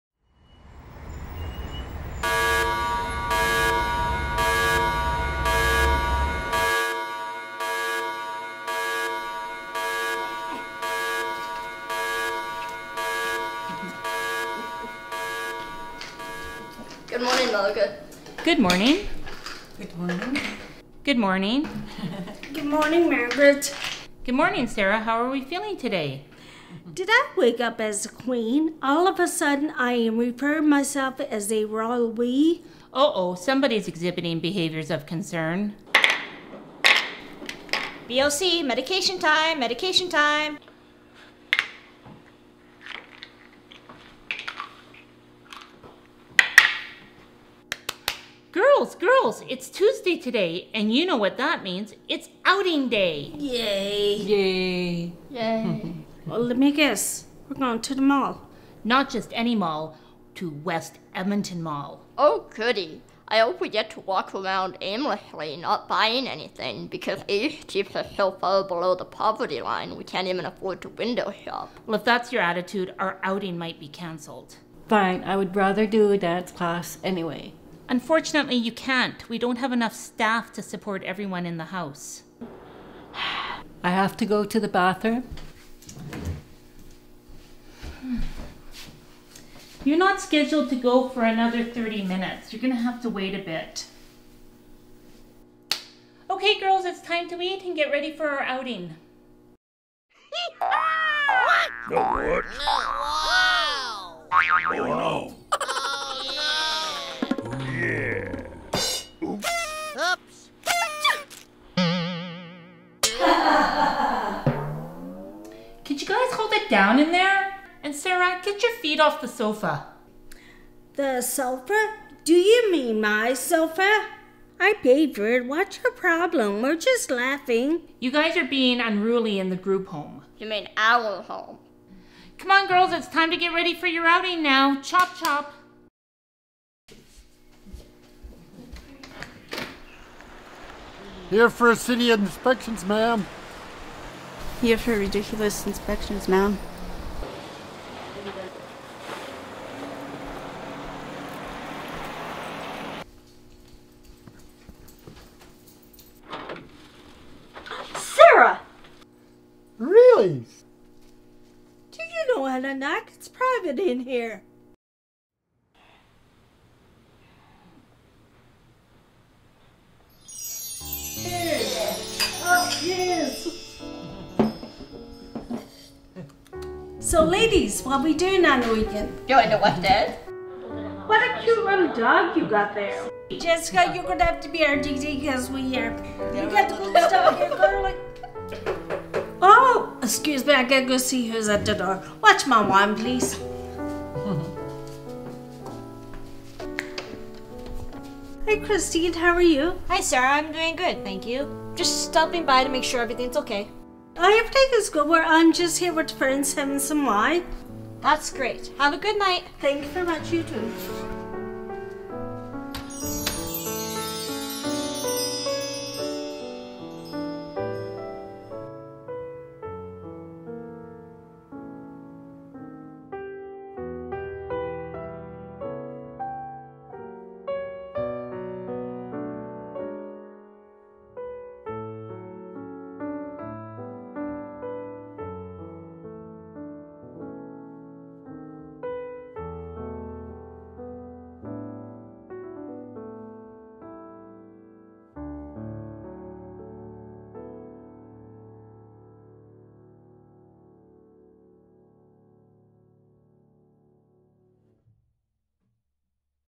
House of Horrors
Written by Chris McBain and Amy Park

This film is meant to provoke discussion about how some staff work with PwD.   This film was written by and for self advocates who met and provided feedback to the writer and the filmer about how they are sometimes treated in group living.